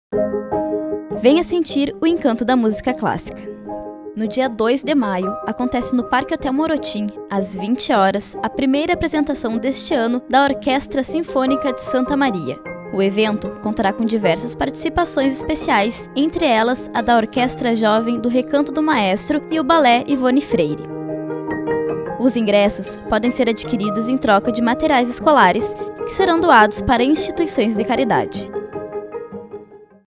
Concerto Orquestra Sinfônica - VTs Institucionais
Nesta terça (02/05), a Orquestra Sinfônica realiza o concerto de abertura da temporada.